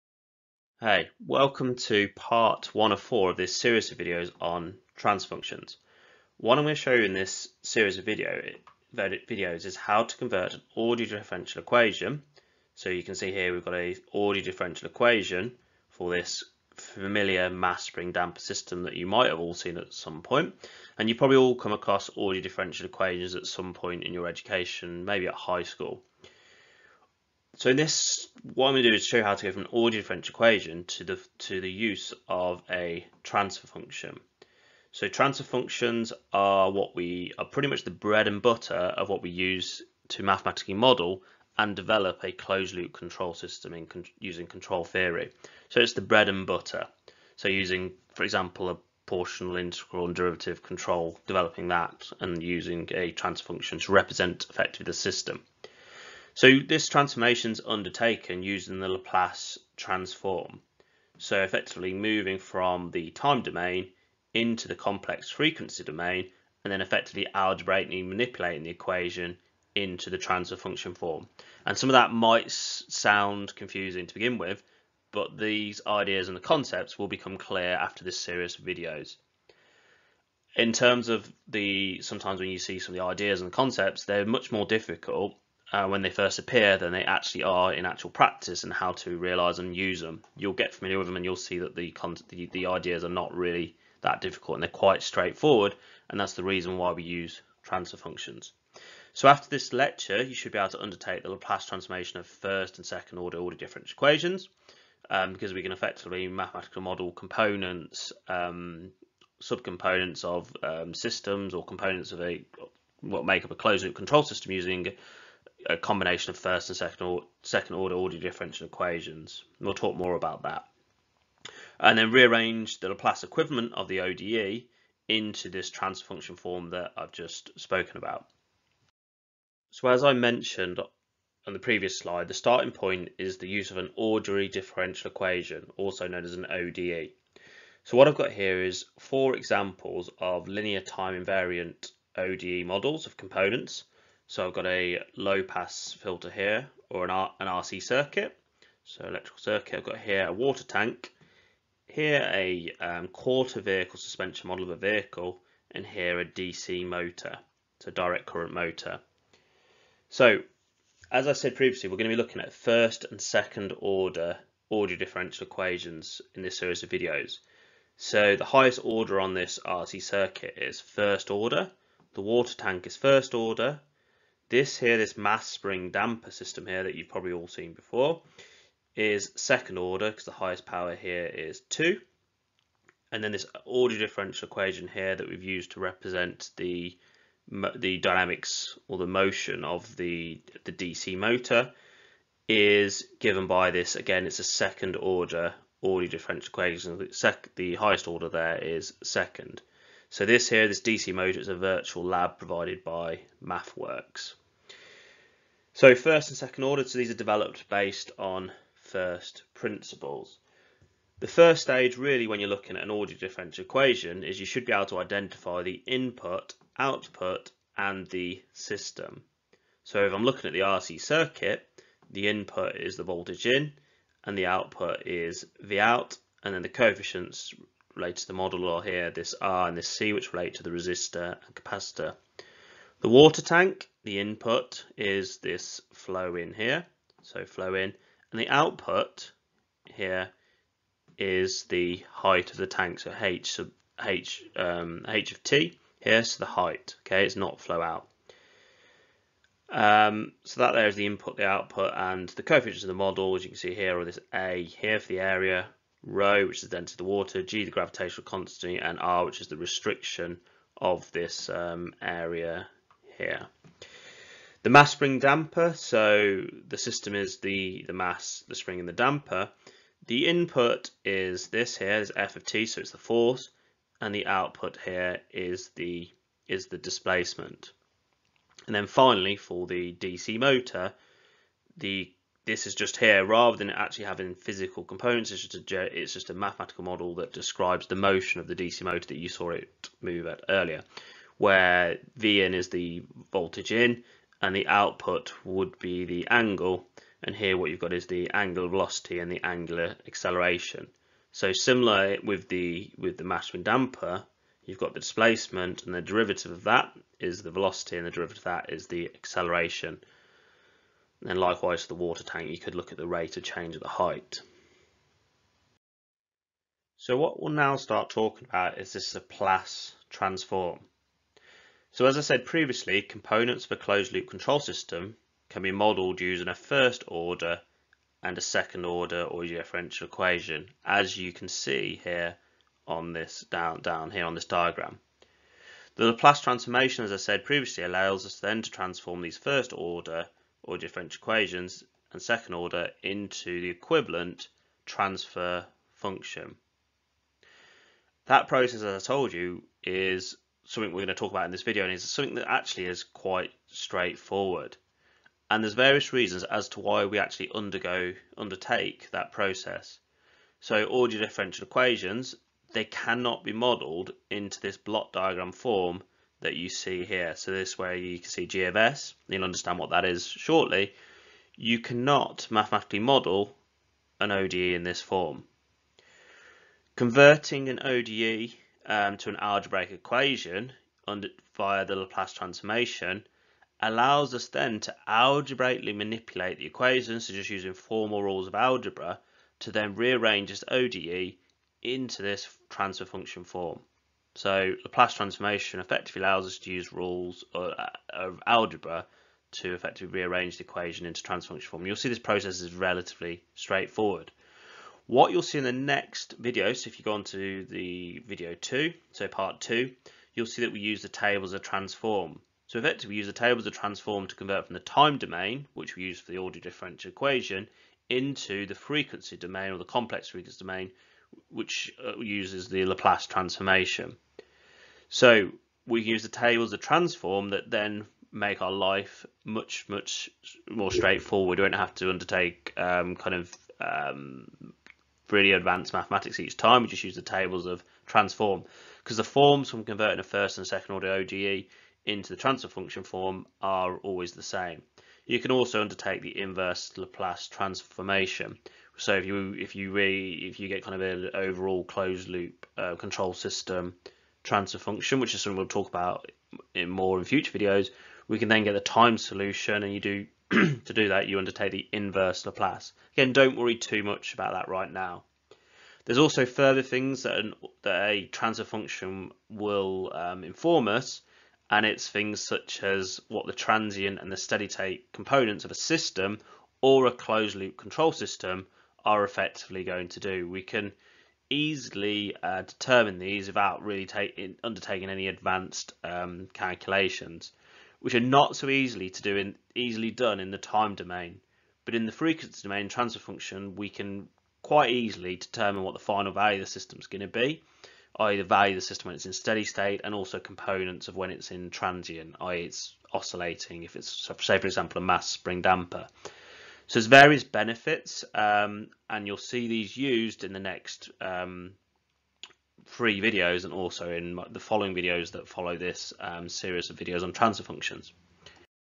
Transfer Functions: Introduction, Part 1 of 4
The four videos on transfer functions detail how to convert an ordinary differential equation (ODE) into a transfer function via the Laplace transformation. Practical examples are given throughout to support the learning.